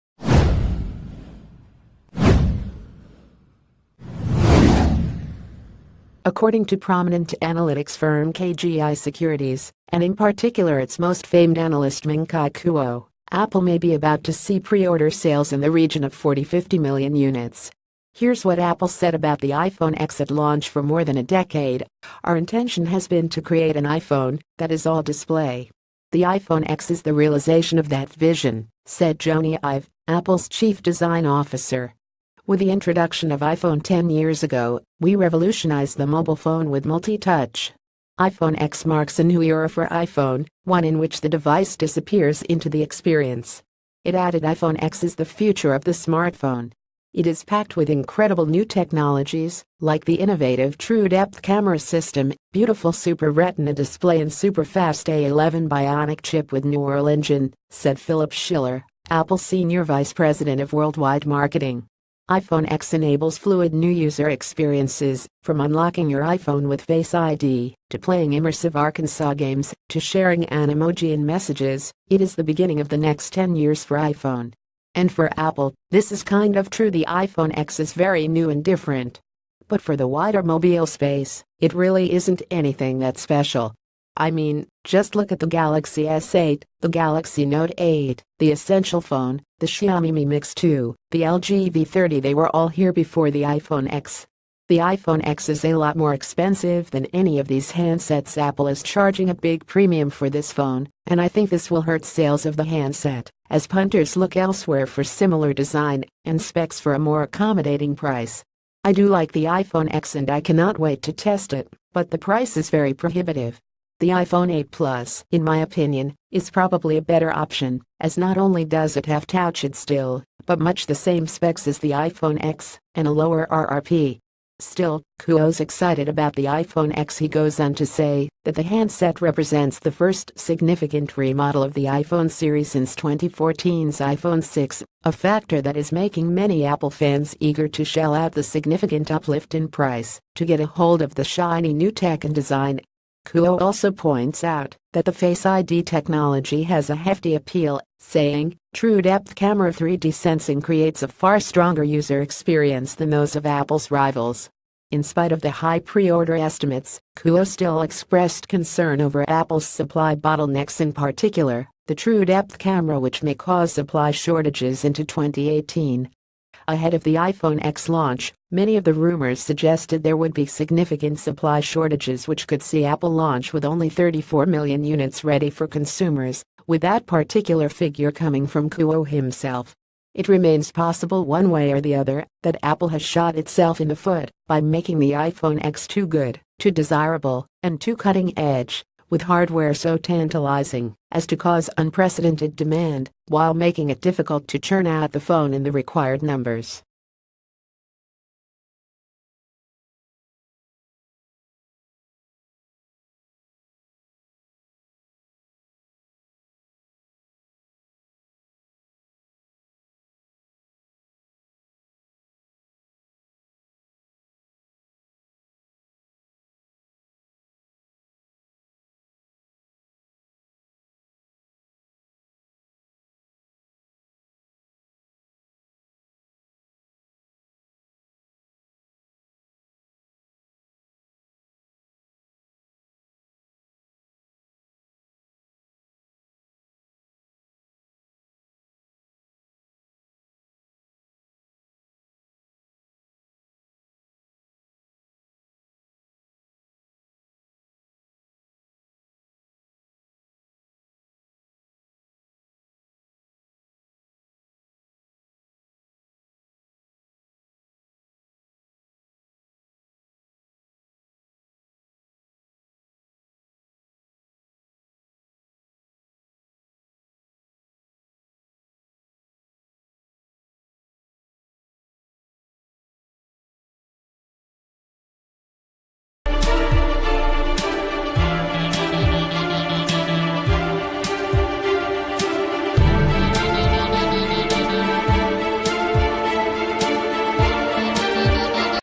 Breaking News | Apple's iphone x preorder to hit 50 million!?
Apple's iphone x preorder to hit 50 million!?

According to prominent analytics firm KGI Securities, and in particular its most famed analyst MingC...